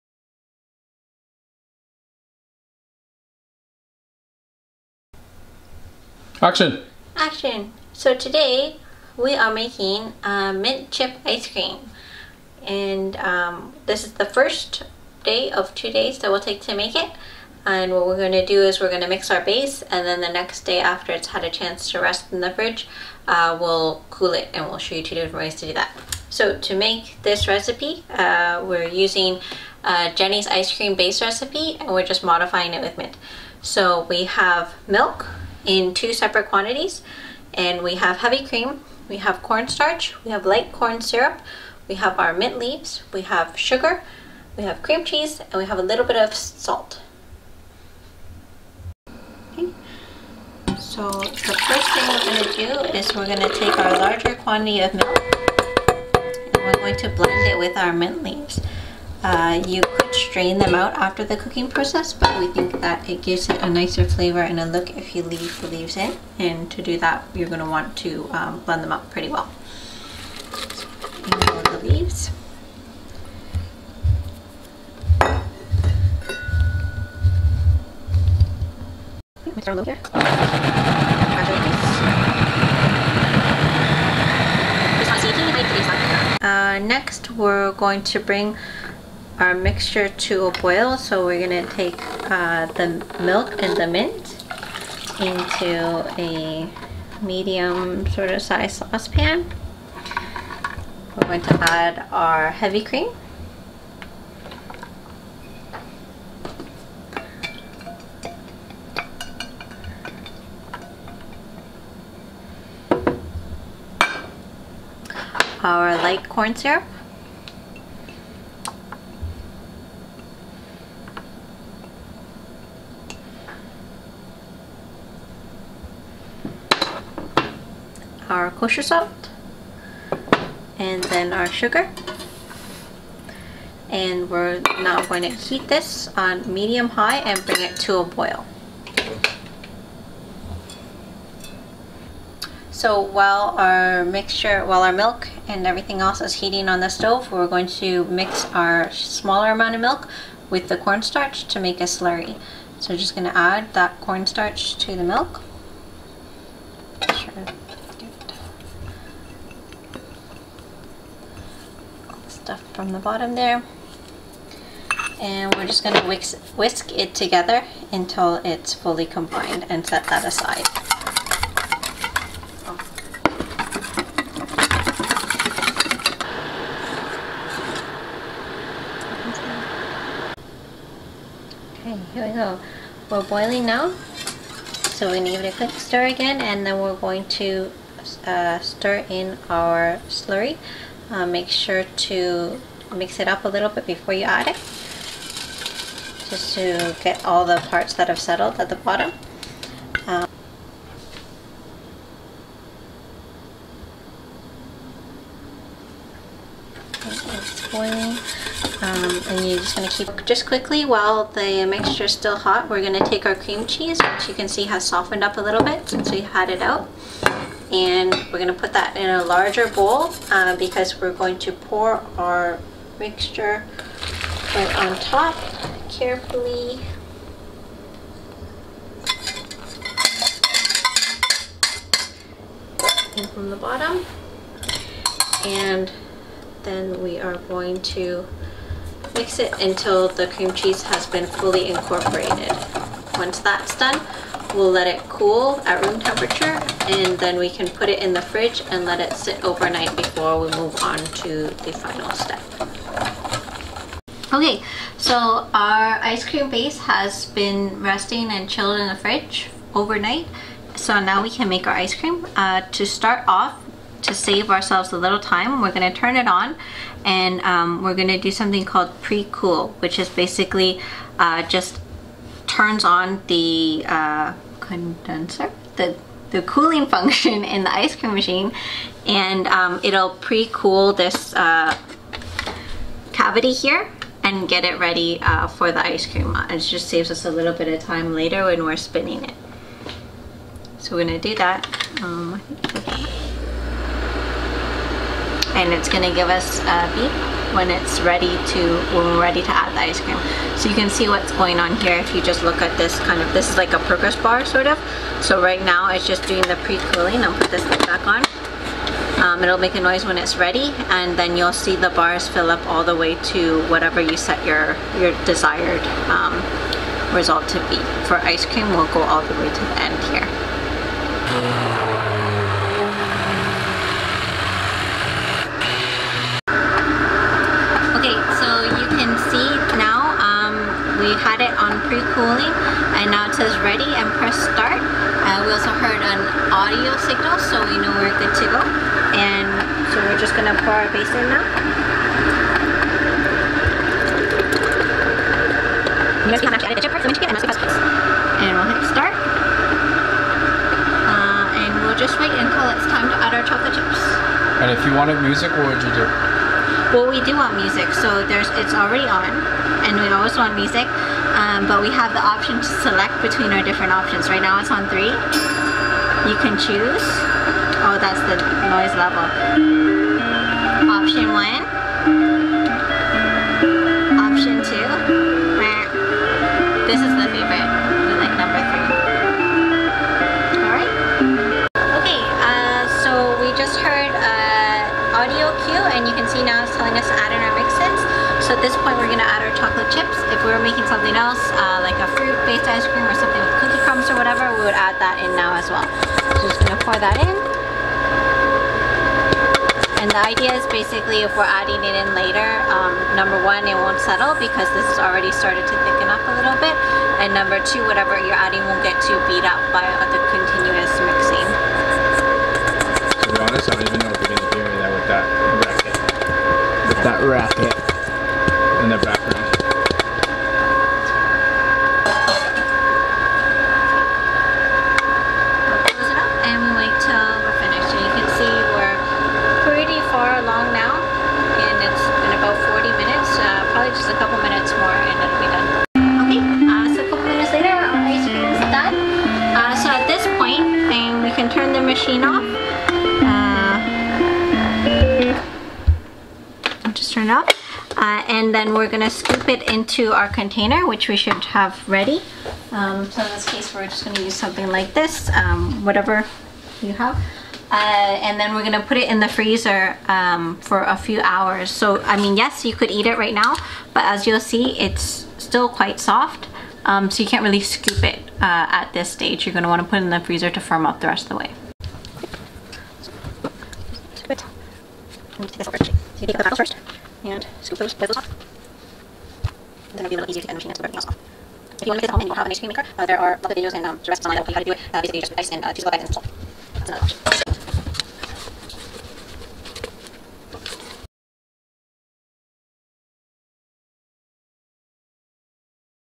Jeni's Ice Cream Base (with Mint and Chocolate Chip)
This is a video on how to make ice cream using the Jeni's Ice Cream Base Recipe from Saveur:

Here, we flavour the base to make a mint chip ice cream.